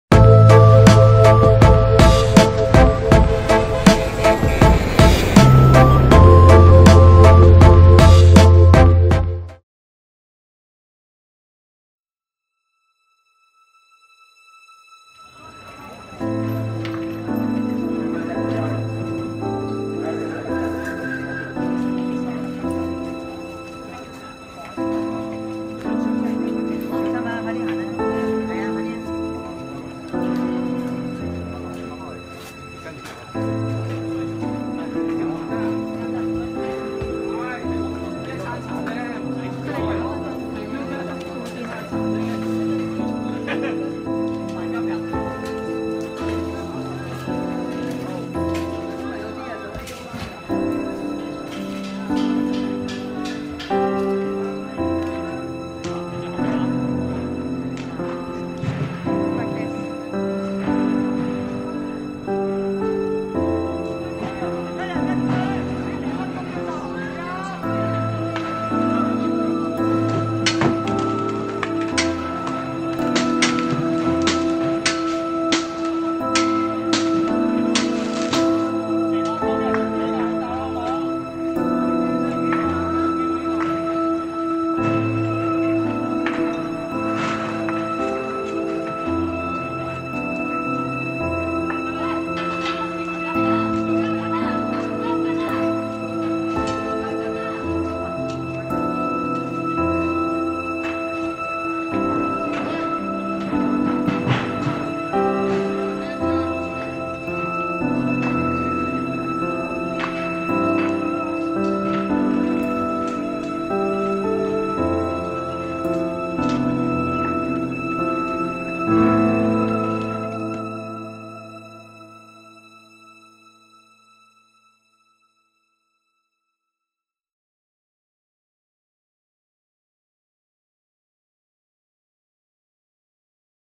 Hong Kong Protests and Riots Part 2
This is part 2 of 3 videos dealing with the Hong Kong protest and riots. Lunar New Year celebrations in Hong Kong turned to chaos as street food vendors clashed violently with police. The clashes are being called the "Fishball Revolution"as the city attempts to crack down down on street food vendors.